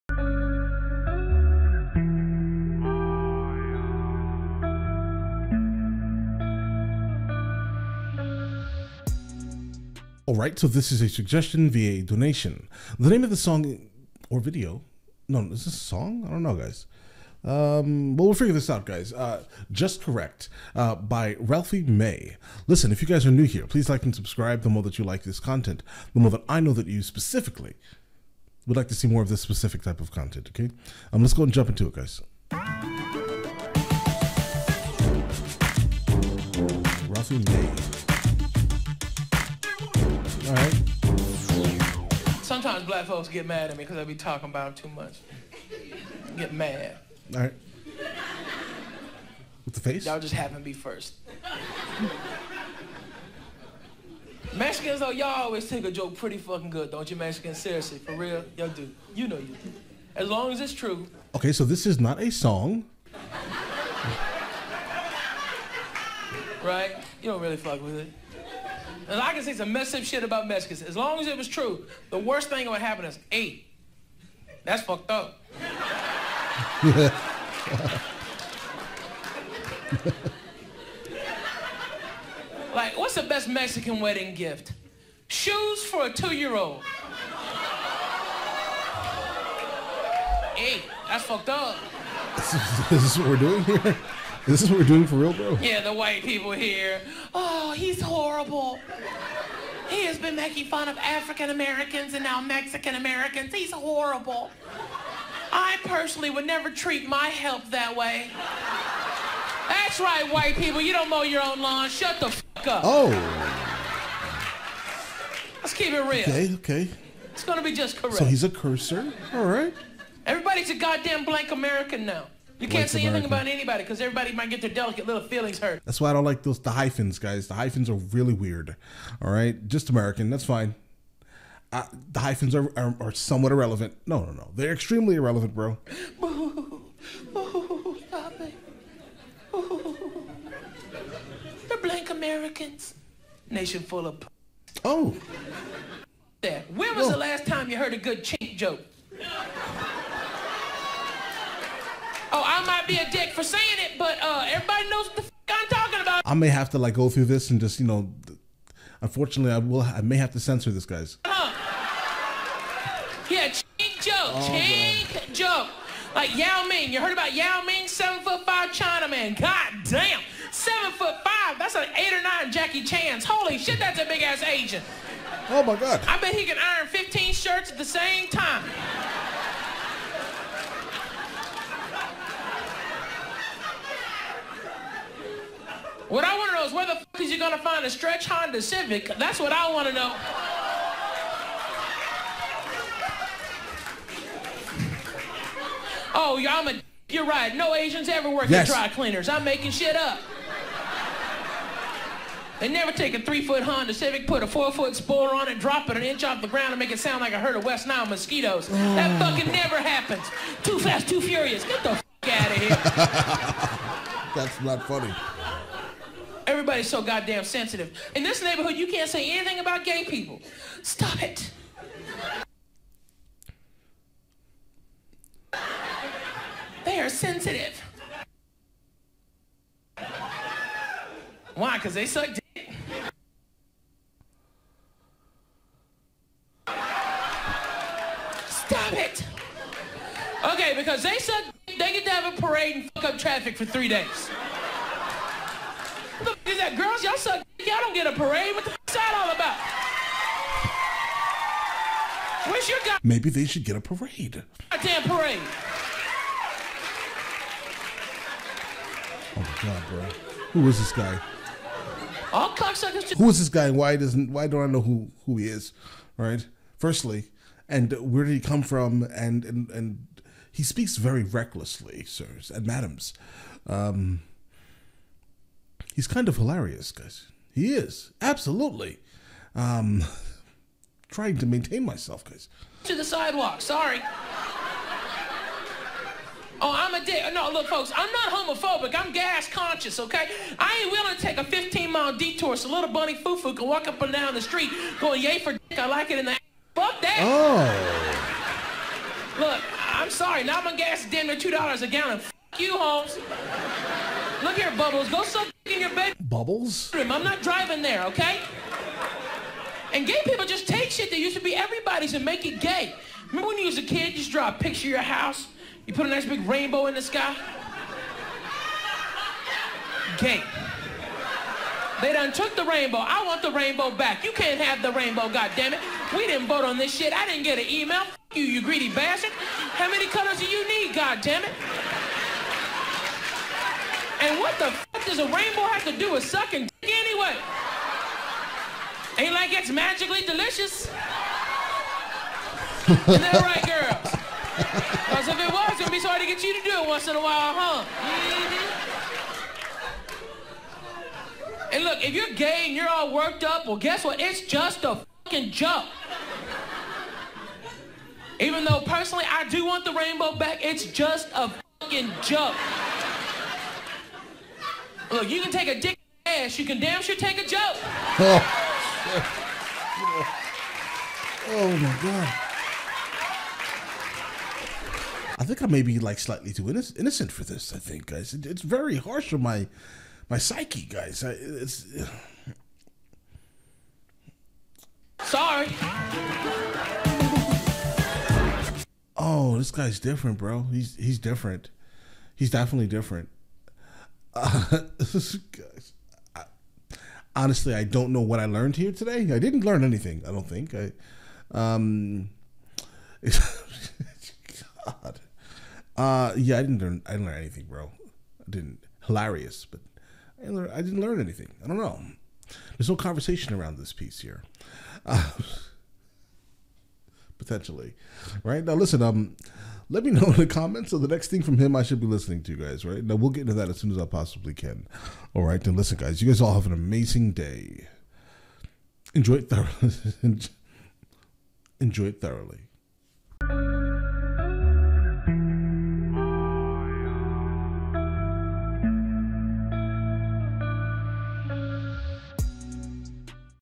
First Time Hearing | Ralphie May - Just Correct Reaction
ralphie may puts a racist audience member on check,